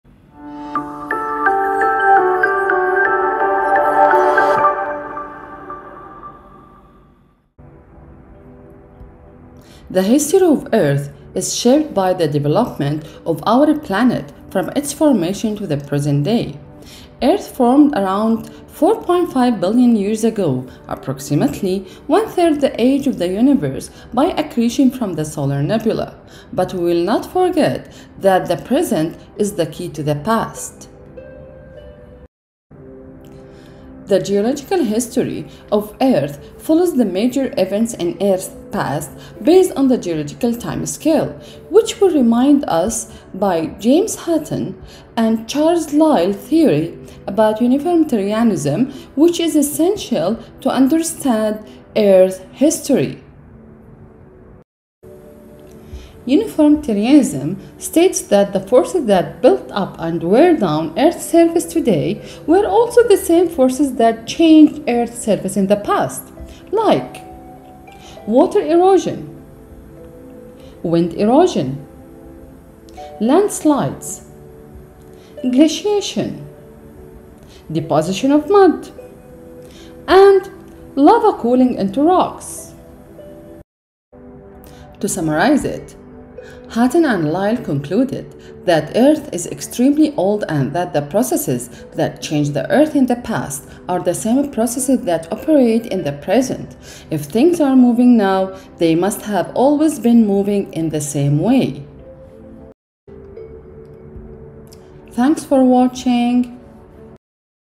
Hutton and Lyell Theories || Mrs. Biology
Hutton and Lyell concluded that Earth is extremely old and that the processes that changed Earth in the past are the same processes that operate in the present.